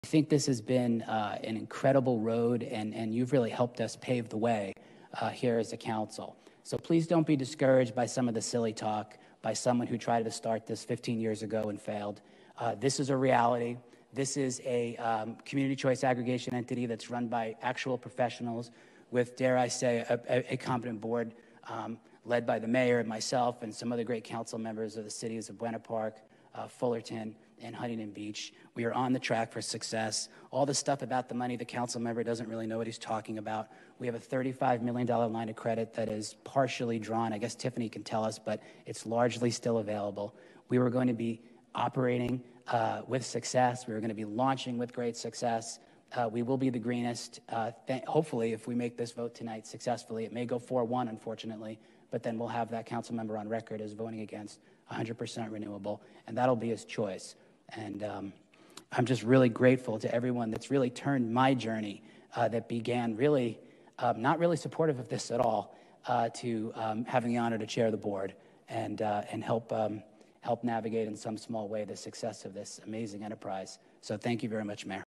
Pt 2 - Mike Carroll Belittles Larry Agran's Questions re the Orange County Power Authority
(Part 2) At the February 8, 2022 Irvine City Council meeting, Mike Carroll belittles the questions and comments raised by Larry Agran regarding the Orange County Power Authority.